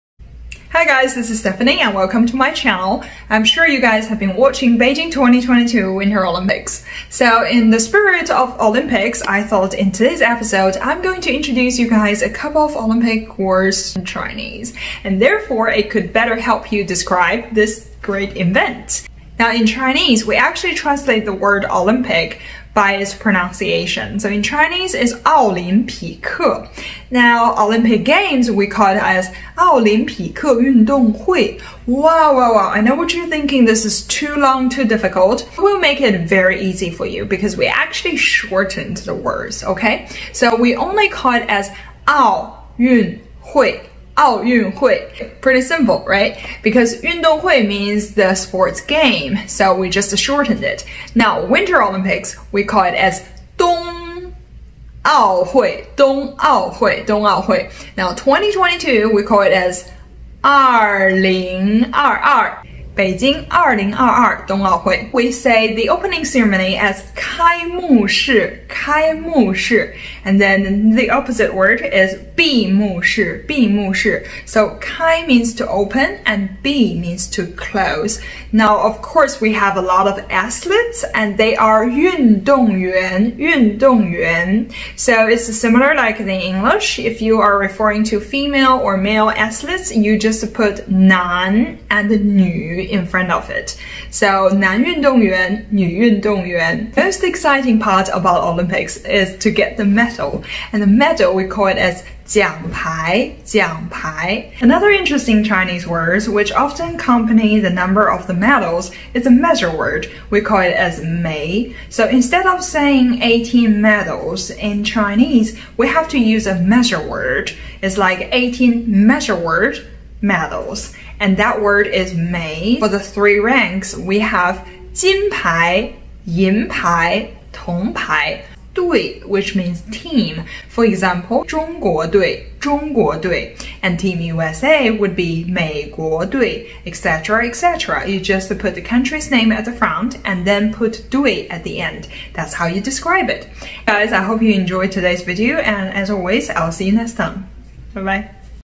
Olympic Games related Chinese words
Hey, hope y'all enjoying the Olympics! Let's learn some Chinese words~